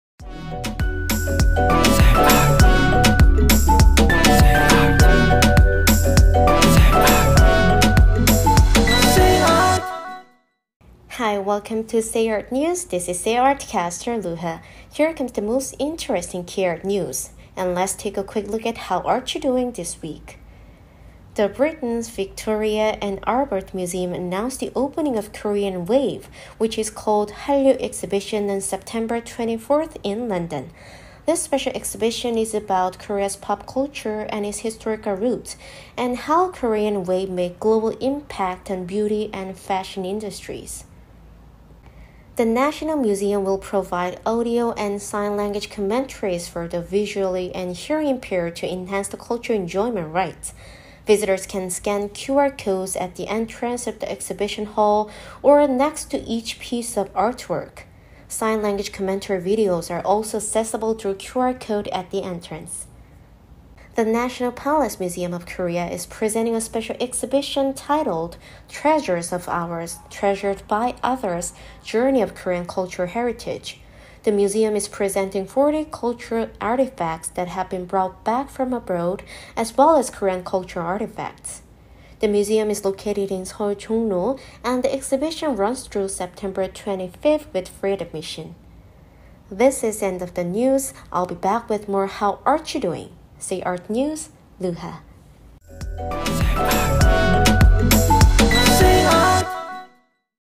[sayart.net] WEEKELY K-Art News Artcaster Luha Lee _3rd Week of July 2022
Hi Welcome to Sayart news this is sayart caster Luha
Here comes the most interesting K-art news.
Let’s take a quick look at “how art you doing this week

The Britain’s Vitoria and Albert museum announced the opening of Korean wave which is called “Hallyu”exhibition on Sept. 24th in London
This special exhibition is about Korea’s pop culture and its historical roots, and how Korean wave made global impact on beauty and fashion industries

The national museum will provide audio and sign language commentaries for the visually and hearing impaired to enhance the cultural enjoyment rights.
Visitors can scan QR codes at the entrance of the exhibition hall or next to each piece of artwork.
Sign language commentary videos are also accessible through QR code at the entrance.

The National Palace Museum of Korea is presenting a special exhibition titled, “Treasures of Ours, Treasured by Others, Journey of Korean Cultural Heritage.  The museum is presenting 40 cultural artifacts that have been brought back from abroad as well as Korean cultural artifacts.
The museum is located in Seoul Jongro and the exhibition runs through Sept. 25th.  with free admission

This is the end of the news
I will be back with more “How art you doing”. Sayart news, Luha